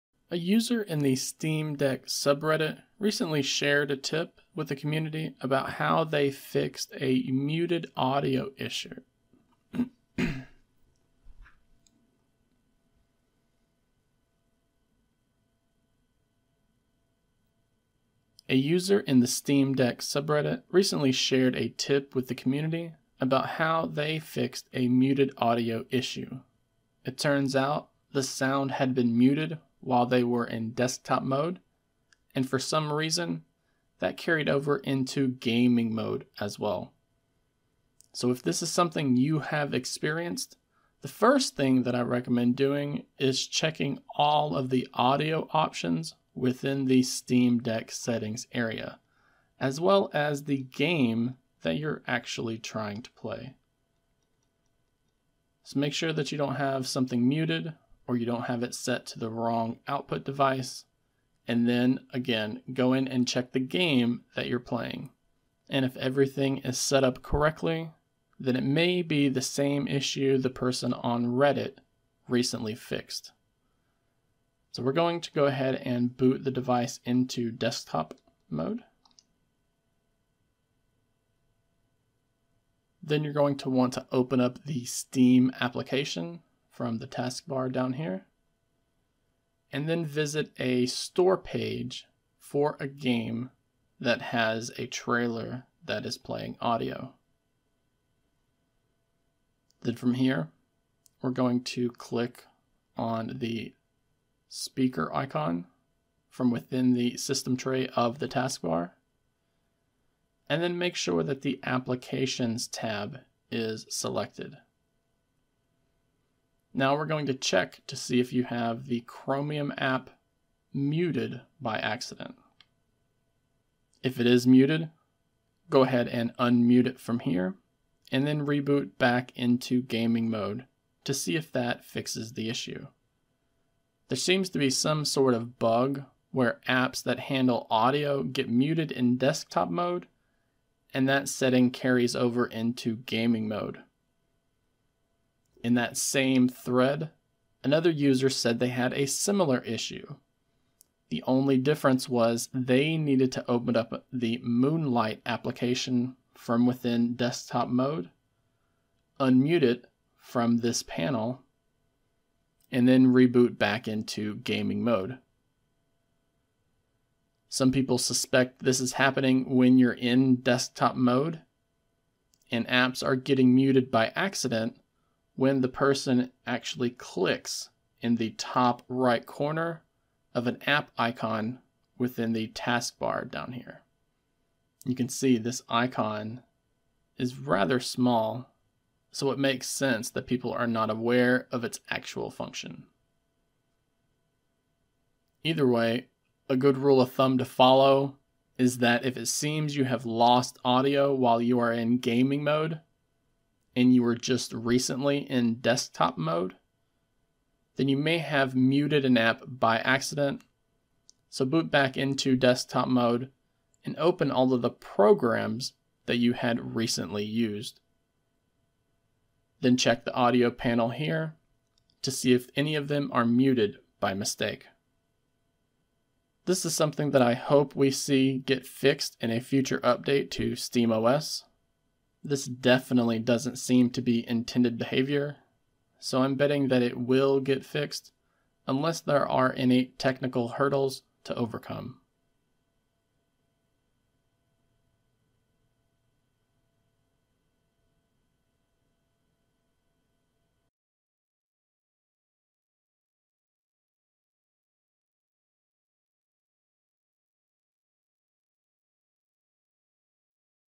Quick Fix for Steam Deck with Muted Audio Issues in Gaming Mode
If you suddenly do not have audio while playing a game, checking some audio options for a few apps in Desktop Mode could be the fix.

Glass Screen Protector
TPU Case
Carrying Case
Portable Power Bank

Summary
A user in the Steam Deck subreddit recently shared a tip with the community about how they recently fixed a muted audio issue. It turns out the sound had been muted while they were in Desktop Mode and for some reason it carried over into Gaming Mode as well.

So if this is something you have experienced, the first thing I recommend doing is checking all of the audio options within the Steam Deck settings area as well as the game you're trying to play. If it seems to be a widespread issue then let's go ahead and boot over into Desktop Mode.

From here, open up the Steam application, head to a game page with a trailer that has audio.
Then click the Speaker icon within the system tray of the task bar.
Now we're going to click the Application tab at the top.
And check to see if the Chromium app has been muted by accident.

If it is muted, unmute it from here and then reboot back into Gaming Mode to see if that fixes it.
There seems to be some sort of bug where apps that handle audio get muted in Desktop Mode and it carriers over into Gaming Mode.

In that same thread, another user said they had a similar issue.
The only difference was they needed to open the Moonlight application in Desktop Mode.
Unmute it from this panel.
And then reboot back into Gaming Mode.

Some people suspect this is happening when you're in Desktop Mode.
And apps are getting muted by accident when they click on the top-right corner of an app icon within the task bar while it's playing audio. You can tell the icon indicating this is rather small so it makes sense that people are not aware of its actual function.

Either way, a good rule of thumb to follow is that if it seems like you've lost audio in Gaming Mode.
And you were just recently in Desktop Mode.
Then you may have muted an app by mistake so boot back into Desktop Mode and open all of the programs you had recently used.

Then check the audio panel to see if any of them are muted by mistake.
Hopefully we see this fixed in a future update to SteamOS.

This doesn't seem to be intended behavior so I'm betting it will be fixed unless there are any technical hurdles to overcome.